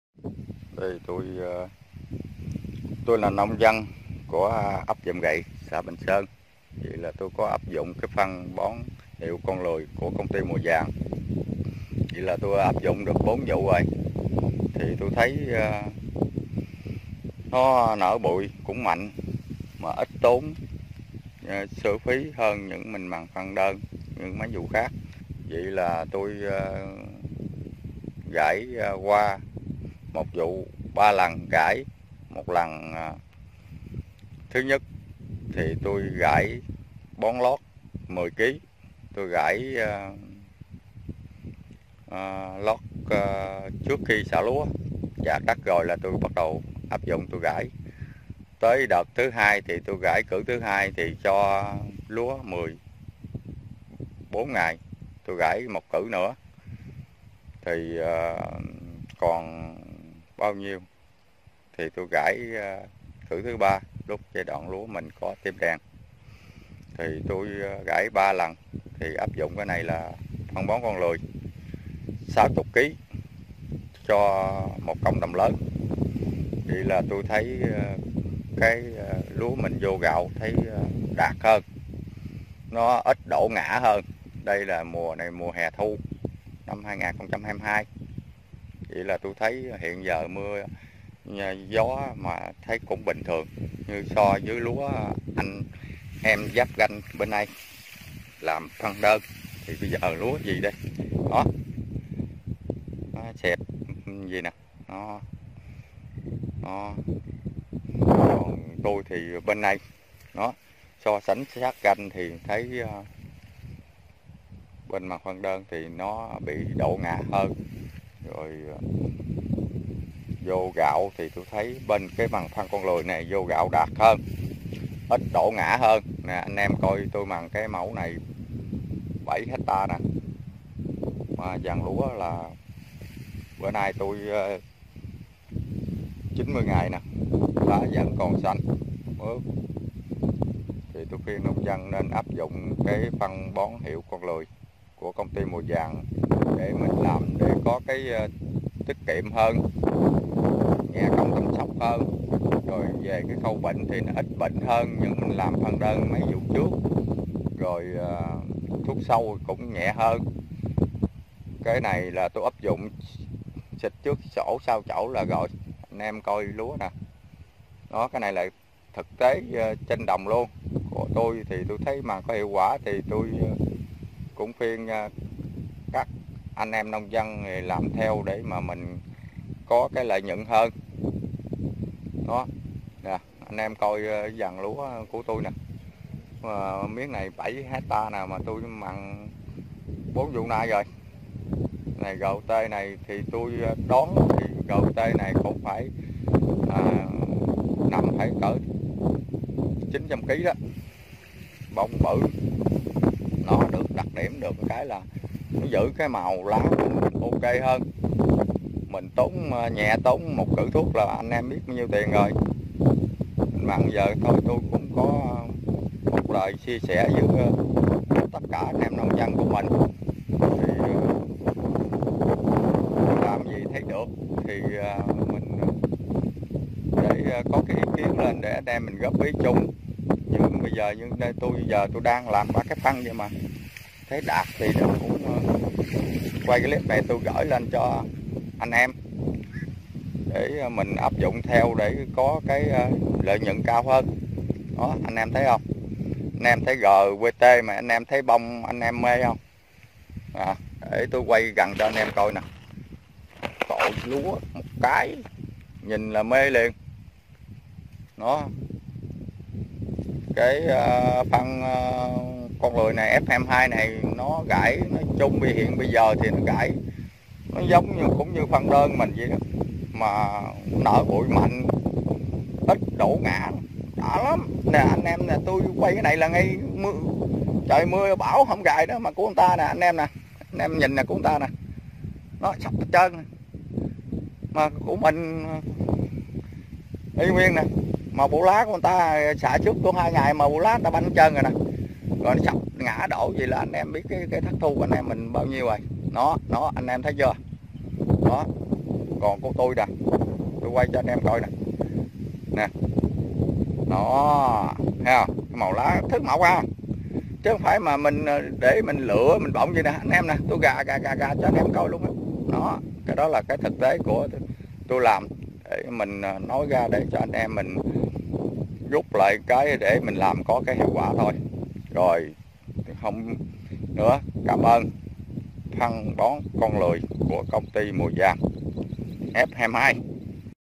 PHÂN BÓN HIỆU CON LƯỜI F22 CHỨNG MINH HIỆU QUẢ VƯỢT TRỘI TRÊN 7HA LÚA RVT TẠI HÒN ĐẤT - KIÊN GIANG
💥💥💥 Chia sẻ của anh Cường tại xã Bình Sơn, huyện Hòn Đất, tỉnh Kiên Giang về quy trình bón và những ưu điểm vượt trội của phân bón hiệu Con Lười sau 4 vụ liên tiếp sử dụng.
👉👉 Ghi hình ngày 28/8/2022 tại 7ha Lúa RVT giai đoạn sắp thu hoạch của anh Cường.